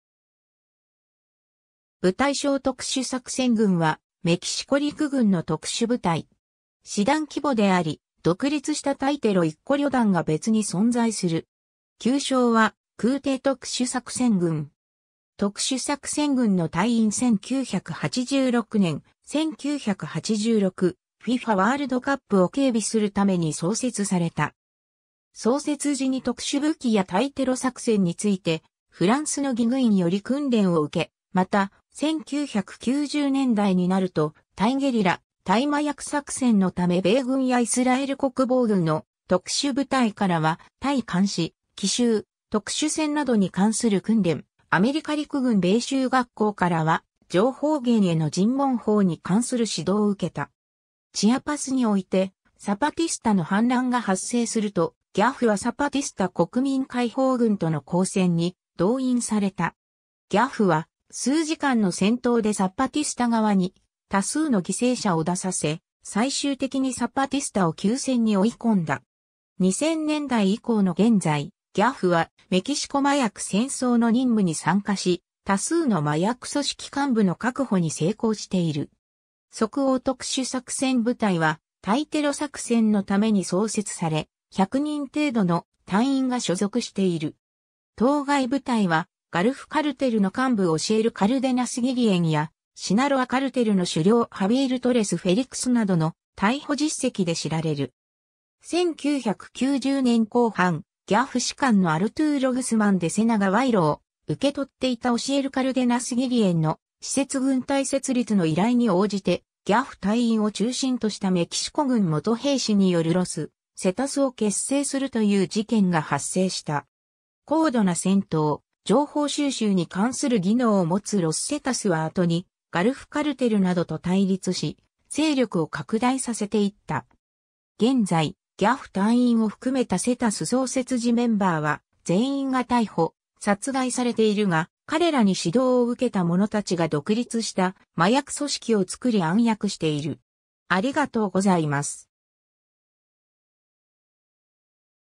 特殊作戦群 (メキシコ軍)
部隊章 特殊作戦群（とくしゅさくせんぐん、スペイン語: Cuerpo de Fuerzas Especiales、略称: GAFE）は、メキシコ陸軍の特殊部隊。
師団規模（2個旅団）であり、独立した対テロ1個旅団が別に存在する。
旧称は空挺特殊作戦群（Grupo Aeromóvil de Fuerzas Especiales）。
 特殊作戦群の隊員 1986年、1986 FIFAワールドカップを警備するために創設された。
創設時に特殊武器や対テロ作戦についてフランスのGIGNより訓練を受け、また、1990年代になると対ゲリラ、対麻薬作戦のため米軍やイスラエル国防軍の特殊部隊からは対監視、奇襲、特殊戦などに関する訓練、アメリカ陸軍米州学校からは情報源への尋問法に関する指導を受けた。
 チアパスにおいてサパティスタの反乱が発生すると、GAFEはサパティスタ国民解放軍との交戦に動員された。
GAFEは数時間の戦闘でサパティスタ側に多数の犠牲者を出させ、最終的にサパティスタを休戦に追い込んだ。
 2000年代以降の現在、GAFEはメキシコ麻薬戦争の任務に参加し、多数の麻薬組織幹部の確保に成功している。
 即応特殊作戦部隊（Fuerza Especial de Reaccion、旧: 空挺特殊作戦群高等コマンド部隊、Grupo Aeromóvil de Fuerzas Especiales del Alto Mando）は、対テロ作戦のために創設され、100人程度の隊員が所属している。
 当該部隊はガルフ・カルテルの幹部オシエル・カルデナス・ギリェンや、シナロア・カルテルの首領ハビエル・トレス・フェリクスなどの逮捕実績で知られる。
 1990年後半、GAFE士官のアルトゥーロ・グスマン・デセナが賄賂を受け取っていたオシエル・カルデナス・ギリェンの私設軍隊設立の依頼に応じて、GAFE隊員を中心としたメキシコ軍元兵士によるロス・セタスを結成するという事件が発生した。
 高度な戦闘・情報収集に関する技能を持つロス・セタスは後にガルフ・カルテルなどと対立し、勢力を拡大させていった。
現在、GAFE隊員を含めたセタス創設時メンバーは全員が逮捕・殺害されているが、彼らに指導を受けた者達が独立した麻薬組織を作り暗躍している。